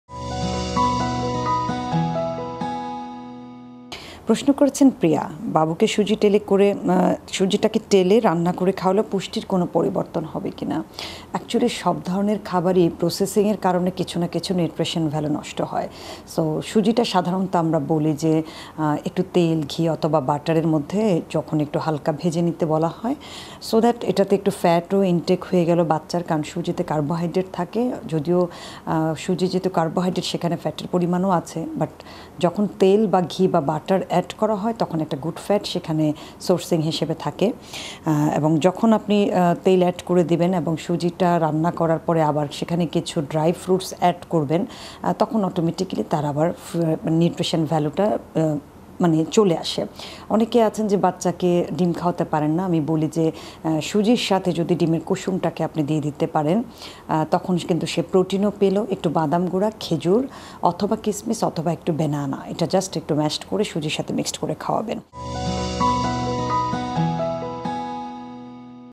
বাচ্চাকে সুজি টেলে রান্না করে খাওয়ালে পুষ্টিগুণ ঠিক থাকে? Nutritionist Aysha Siddika
আপনাদের বিভিন্ন প্রশ্নের উওর জেনে নিন Nutritionist Aysha Siddika এর কাছ থেকে

পুষ্টিবিদ আয়শা সিদ্দিকার চেম্বার:

চেম্বার
ধানমন্ডি